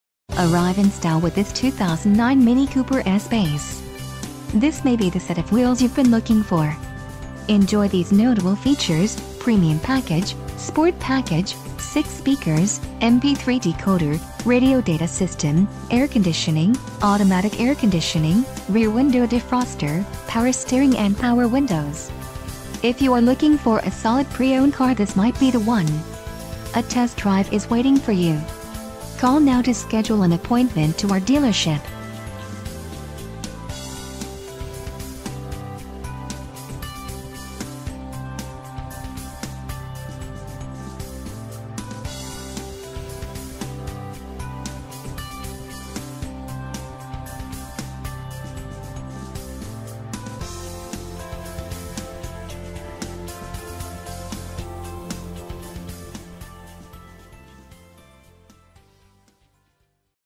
2009 Mini Cooper S Base - Veterans Ford - Tampa, FL 33625
CLEAN CARFAX CHECK NO DAMAGE REPORTED and SUNROOF. Premium Package (Automatic Air Conditioning and Dual-Pane Panoramic Power Sunroof), Sport Package (Dynamic Traction Control), and Bluetooth and USB/iPod Adapter. Put down the mouse because this 2009 Mini Cooper S is the car you've been hunting for. Mini has established itself as a name associated with quality. This Mini Cooper S will get you where you need to go for many years to come. Awarded Consumer Guide's rating as a 2009 Sporty/Performance Best Buy. Nobody beats a Veterans Ford deal!!